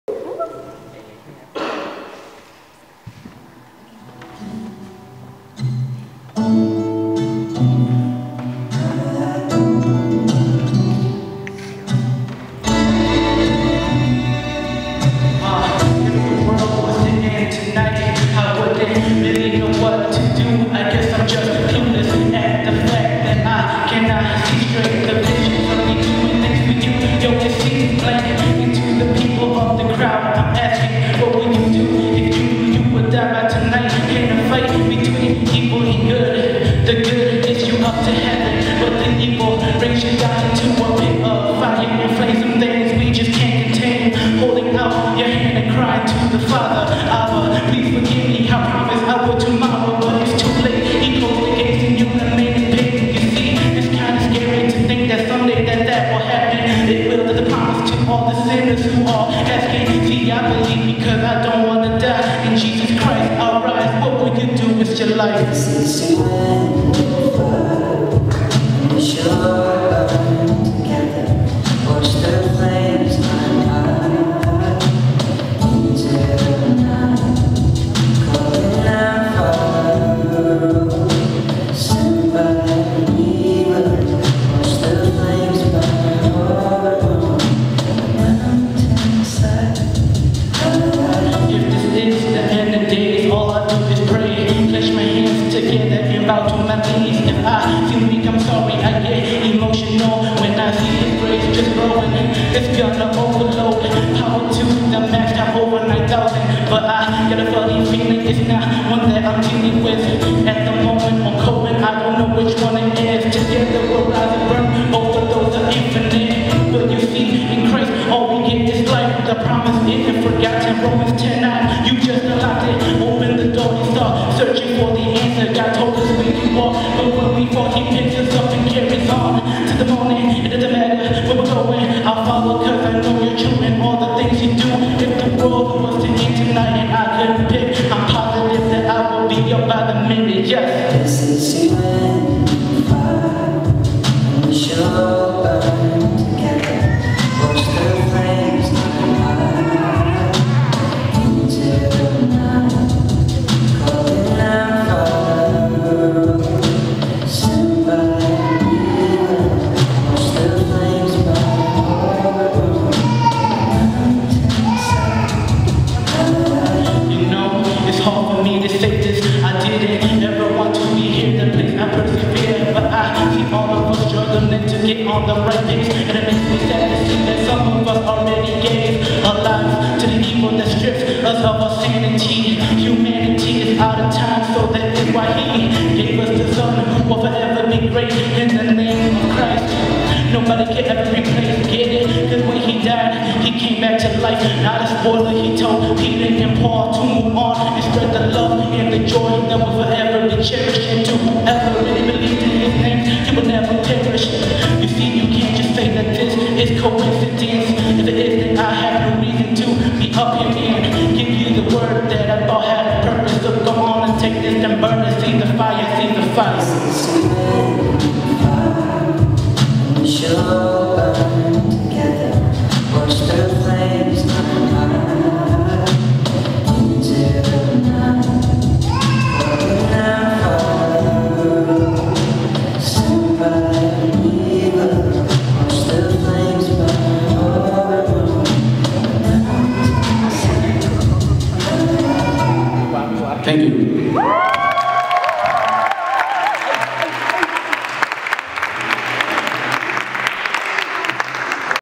iamvyze (Shekinah Benefit Concert '15)
He had 2 days to prepare for the song, hence the reason for reading his lyrics. Pheng Vang aka iamvyze performs at the benefit concert.